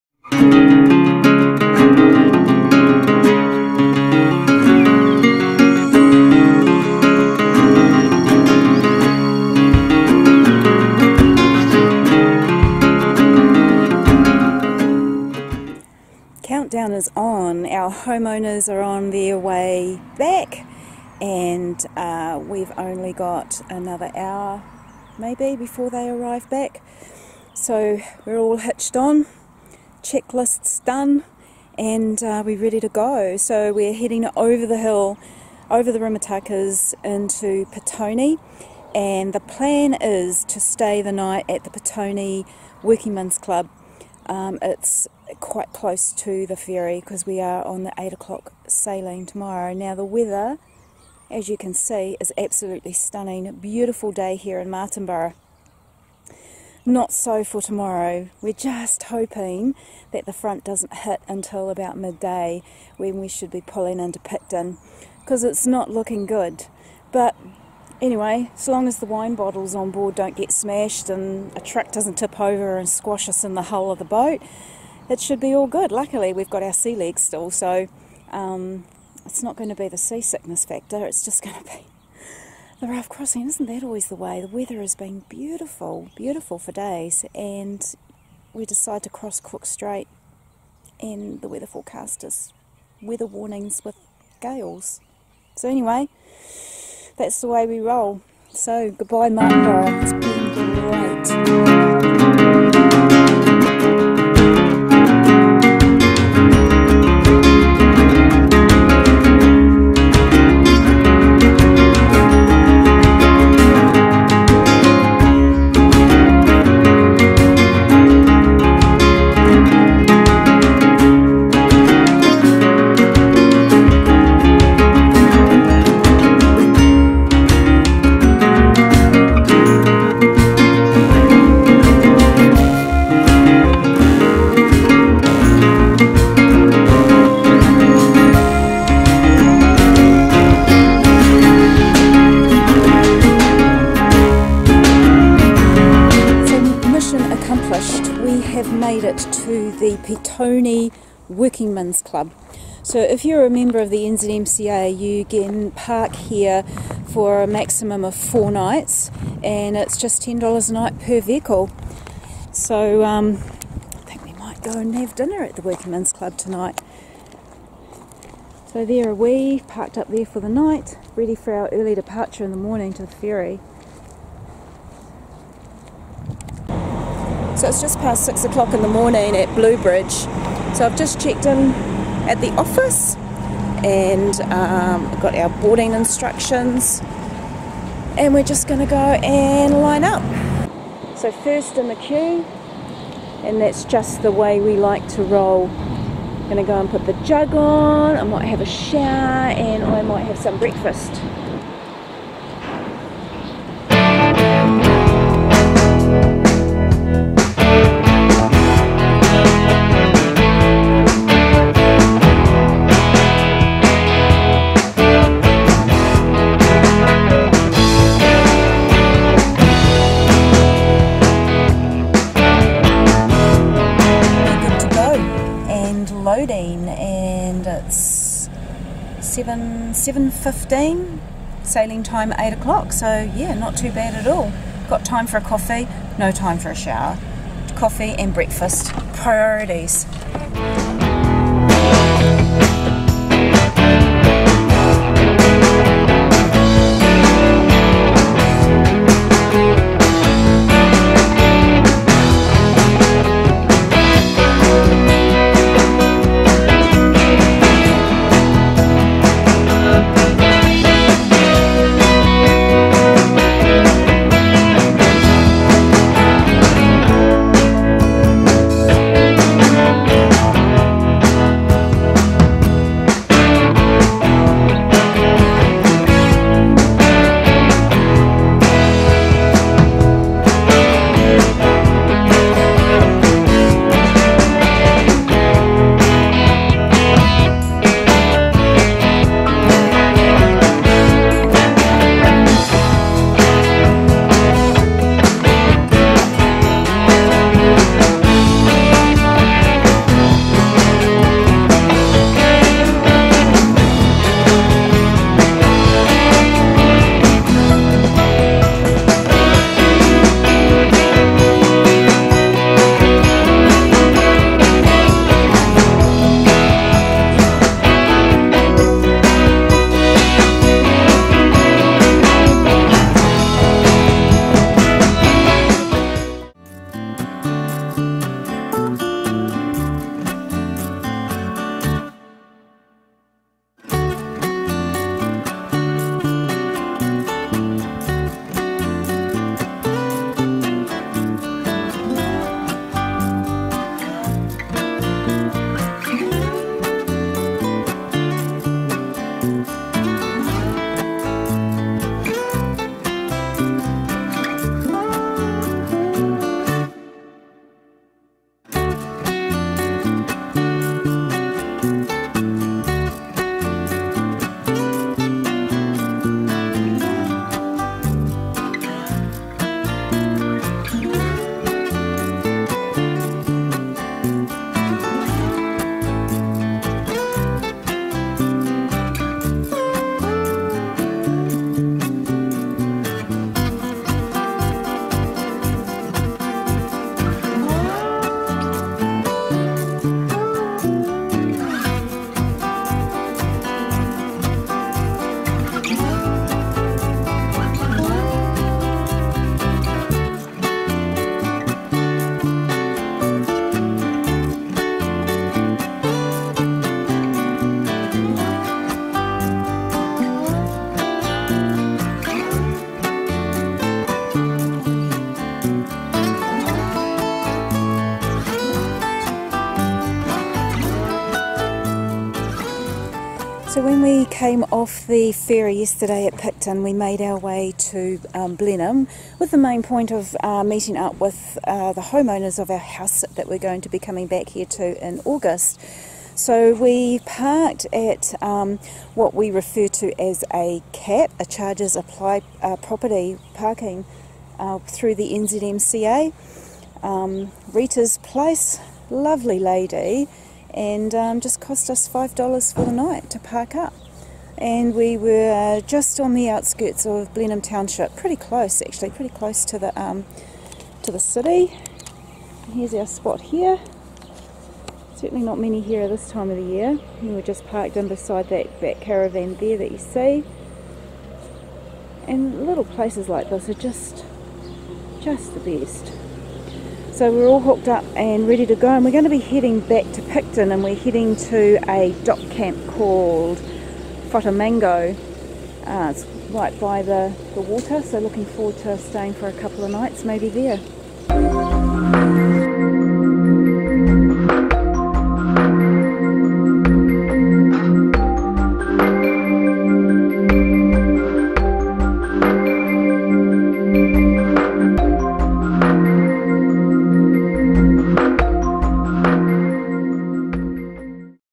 South Island Bound
We're bucking the trend and heading to the South Island as winter descends.  It's okay though, we will hang around the top half of the South Island. It is slightly warmer in those cold winter months than in Central Otago and we have some housesitting assignments in the top half.

We spent the night at Petone, ready for our Cooks Straight crossing with Bluebridge Ferry.